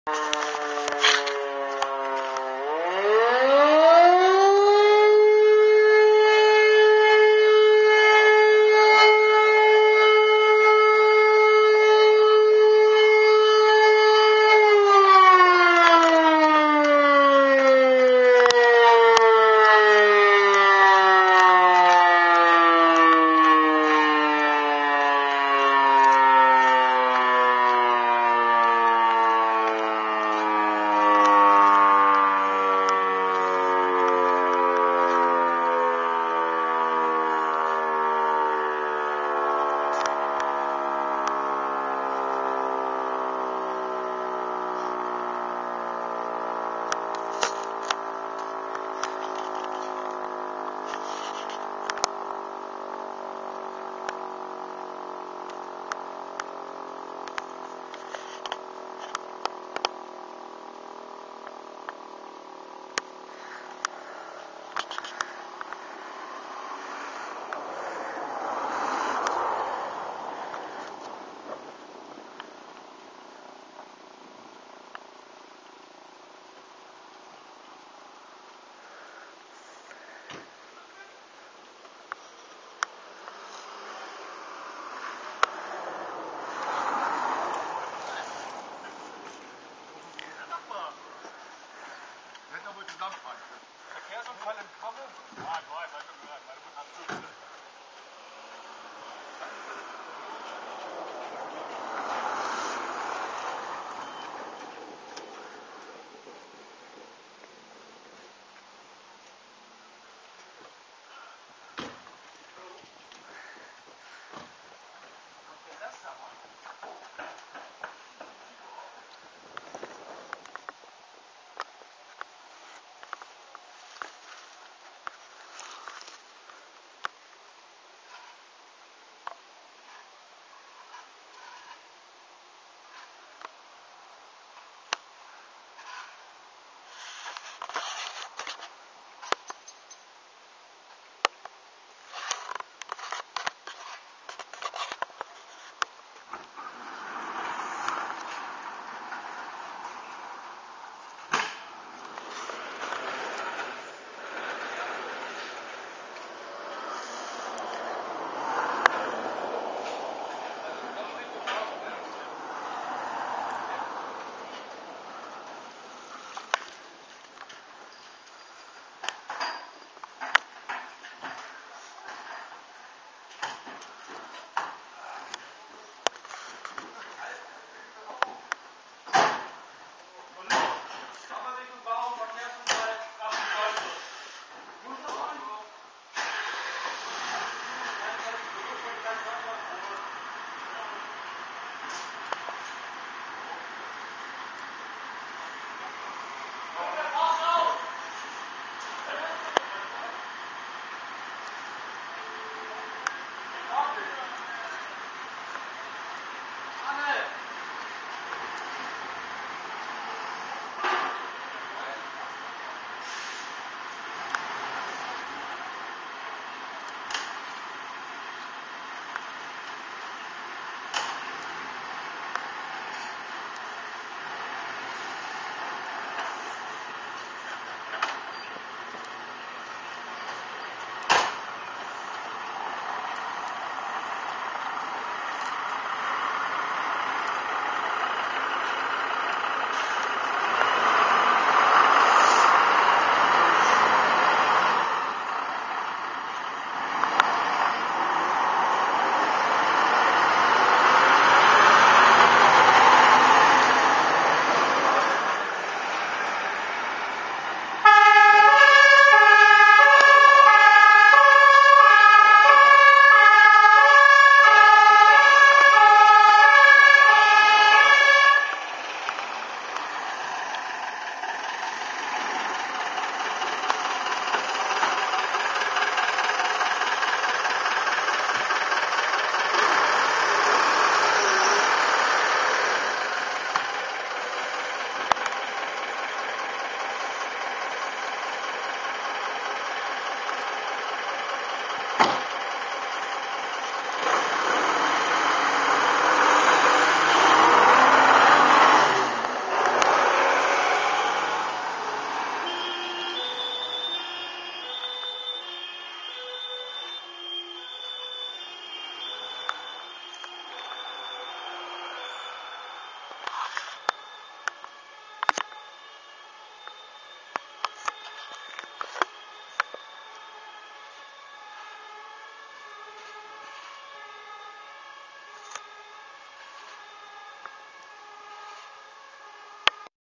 Einsatz FF Groß Flöthe ( Alarmierung+Ausrücken + HD)
Am Mittag des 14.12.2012 um ca. 14 Uhr wird die FF Groß Flöthe zu einem Verkehrsunfall zwischen Cramme und Barum alarmiert. Die Kameraden begeben sich umgehend zum Feuerwehrhaus. Die beiden Fahrzeuge HLF 20/24 und das LF 8/6 der FF Groß Flöthe rücken mit Sonder- und Wegerechten aus.
Beim Verkehrsunfall waren 2 PKW's beteiligt, wobei beide Fahrzeuge stark beschädigt wurden. Im Einsatz waren die BF Salzgitter, die FF Cramme, die FF Groß Flöthe sowie die FF Klein Flöthe. Ebenfalls war auch ein FuStW der Polizei vor Ort ,der die Straße absperrte. Ein besonderer Dank geht an den ersten Kamerad der eintrifft und an den Beifahrer des LF 8/6 für den netten Gruß. :)
Entschuldigt das Wackeln und die sämtlichen Nebengeräusche im Video, da es etwas kalt war.

HLF (Hilfeleistungslöschgruppenfahrzeug) 20/24
LF (Löschgruppenfahrzeug) 8/6
BF (Berufsfeuerwehr)
FuStW (Funkstreifenwagen)